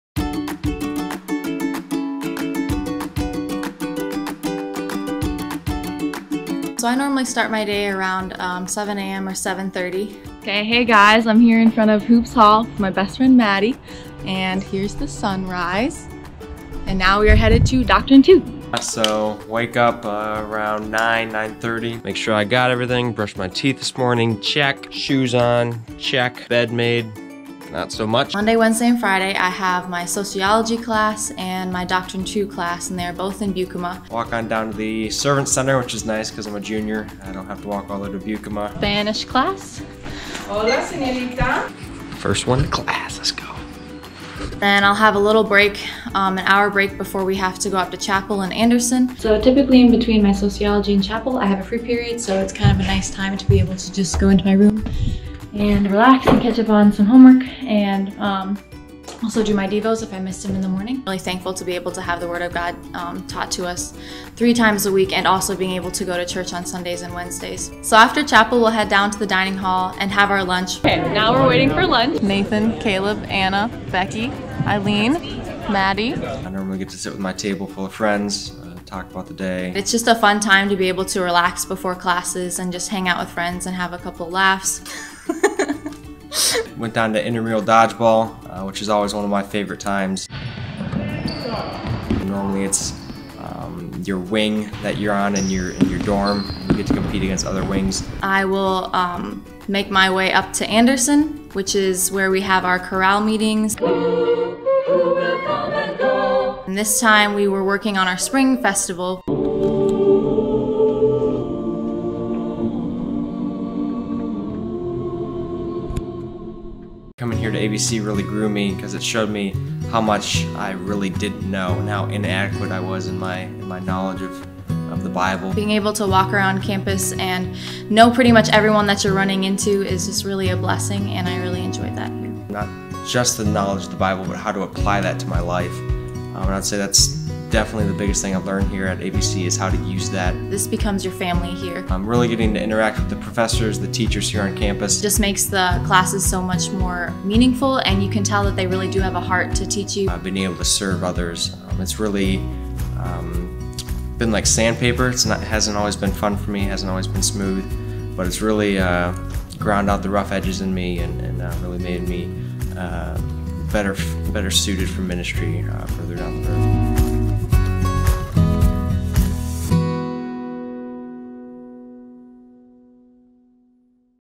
A Day in the Life of an ABC Student
From classes and chapel to lunch and intramurals, Jordan and Albina take us through a day at Appalachian Bible College.
An accredited Bible college in West Virginia, ABC exists by God's grace and for His glory to educate and equip servants for the Church of tomorrow while edifying the Church of today.